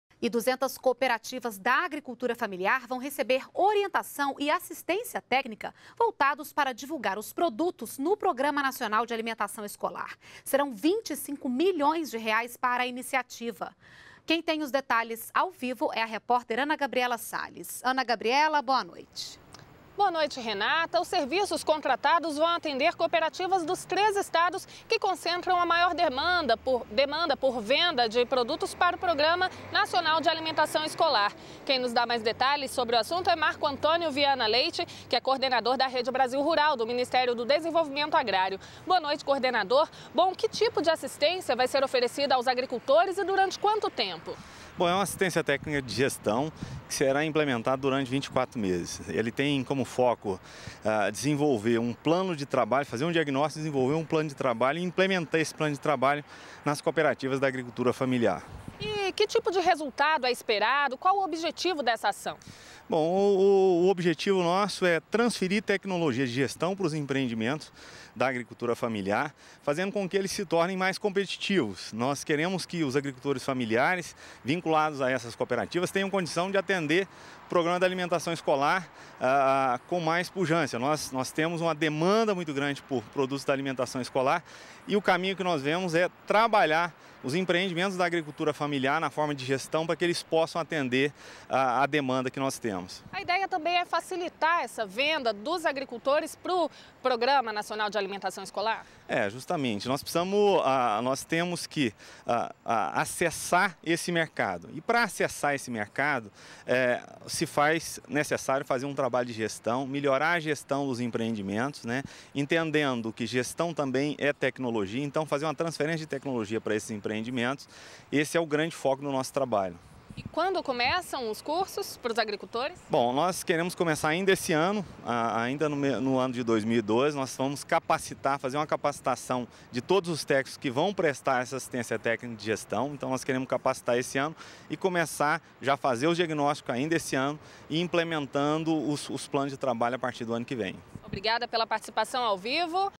Agricultores familiares vão receber orientações para melhorar gestão e divulgação dos produtos
NBR NOTÍCIAS - 08.10.12: Agricultores familiares de cooperativas vão receber orientação técnica para melhorar o sistema de gestão e divulgação dos seus produtos. O objetivo é permitir maior competitivadade do setor para atender o Programa Nacional de Alimentação Escolar. Ao todo, serão 200 cooperativas que vão receber o serviço de orientação. O Minsitério do Desenvolvimento Agrário (MDA) investiu cerca de R$ 25 milhões na ação. Para o coordenador da Rede Brasil Rural, Marco Viana, o foco da assistência técnica é transferir tecnologias de gestão para que os agricultores familiares se tornem mais produtivos. A formação deve começar ainda em 2013.